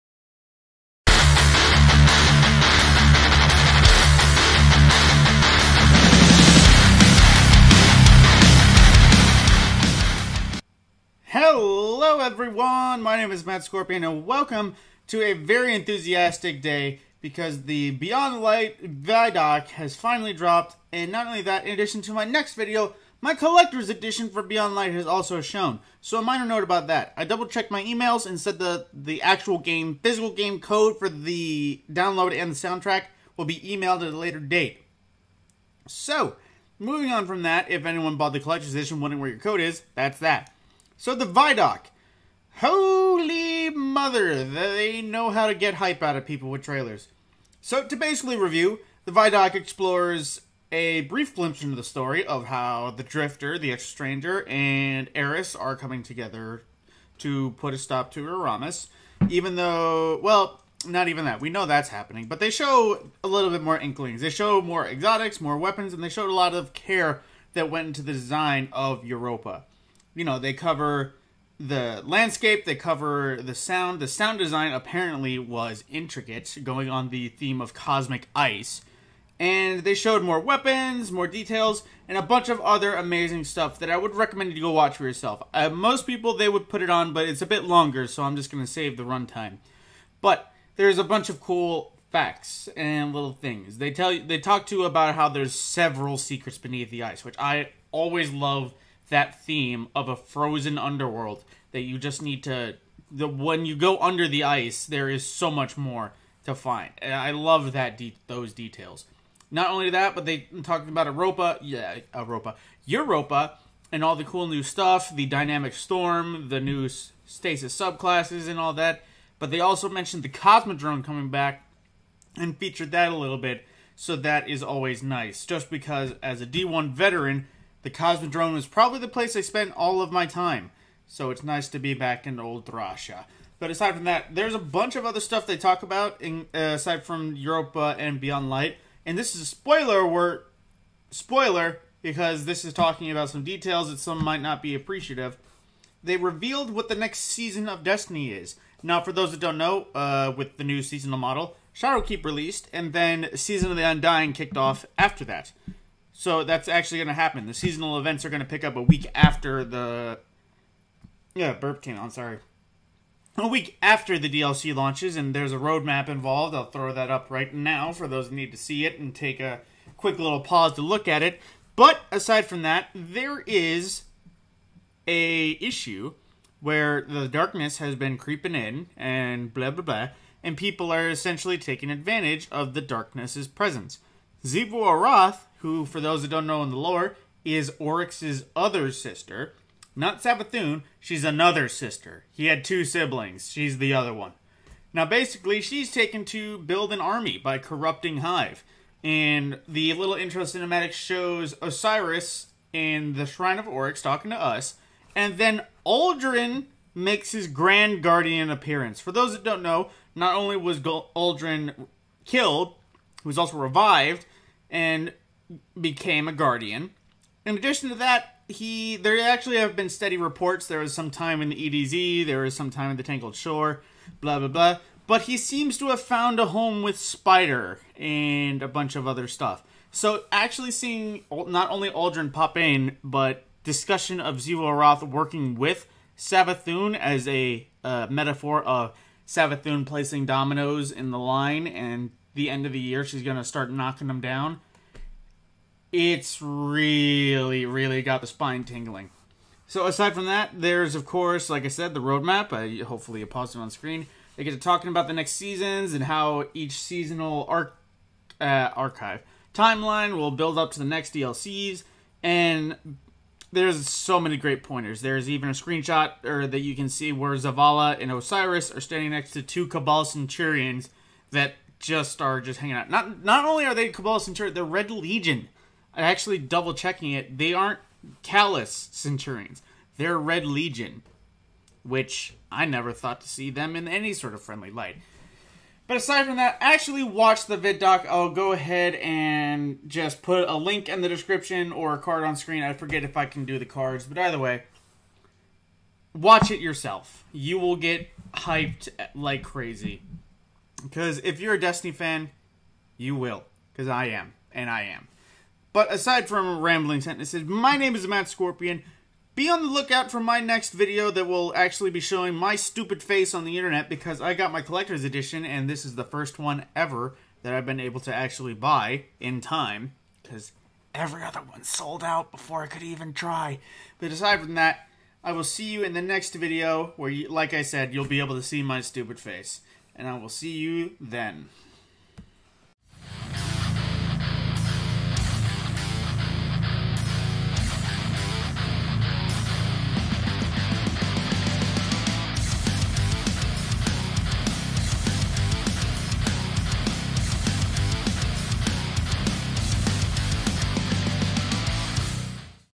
New Bungie ViDoc Brings The Hype Again!!: Destiny 2 ViDoc Review
We got the Beyond Light ViDoc finally! Here is a brief run though of Important pointers for it but here is a link for it to watch it all yourself.